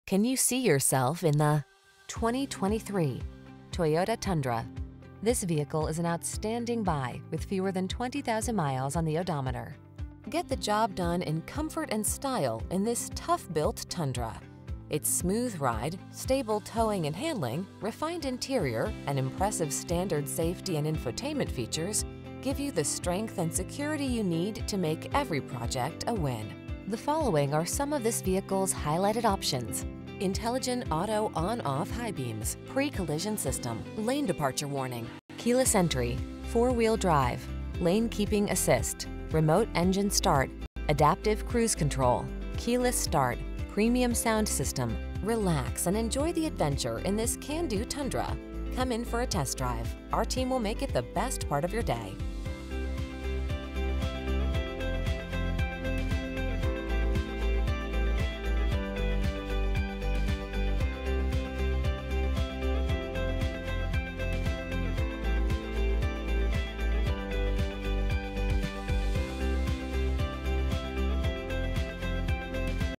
2023 Toyota Tundra_4WD SR5 Mesa, Tempe, Chandler, Scottsdale, Gilbert AZ
Pre-Owned 2023 Toyota Tundra_4WD SR5 available at Berge Toyota located in 2020 W. Riverview Auto Drive, Mesa, AZ, 85201. Servicing the Mesa, Tempe, Chandler, Scottsdale, Gilbert area.

Can you see yourself in the  2023  TOYOTA Tundra.  Get the job done in comfort and style in this tough-built Tundra. Its smooth ride, stable towing and handling, refined interior, and impressive standard safety and infotainment features give you the strength and security you need to make every project a win.
 The following are some of this vehicles highlighted options: Intelligent Auto on/off High Beams, Pre-Collision System, Lane Departure Warning, Keyless Entry, Four Wheel Drive, Lane Keeping Assist, Remote Engine Start, Adaptive Cruise Control, Keyless Start, Premium Sound System. Relax and enjoy the adventure in this can-do Tundra. Come in for a test drive. Our team will make it the best part of your day.